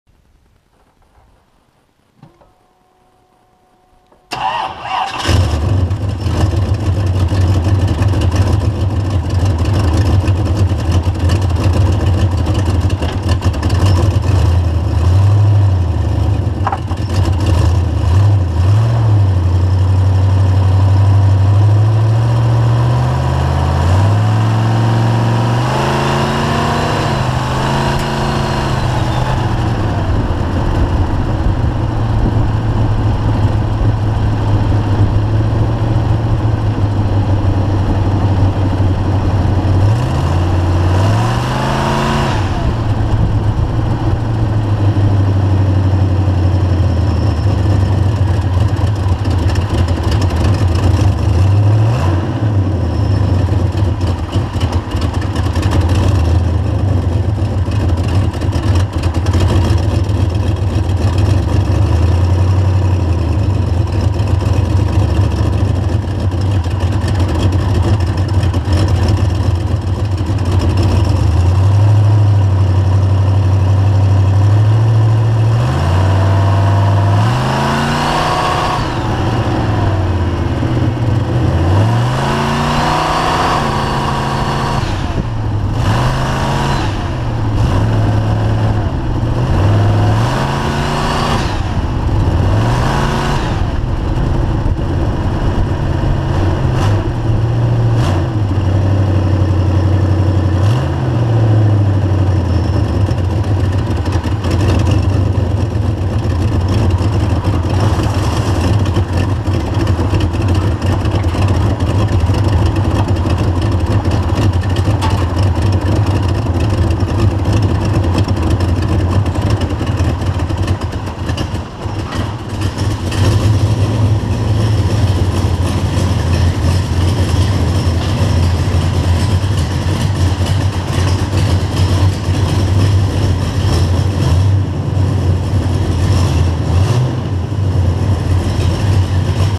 1968 Ford Torino Turbo missing issue 2
Trying to work out some missing issues. Update : just installed a wideband commander.  Air/fuel @ idle was 18 +. Runs much smoother!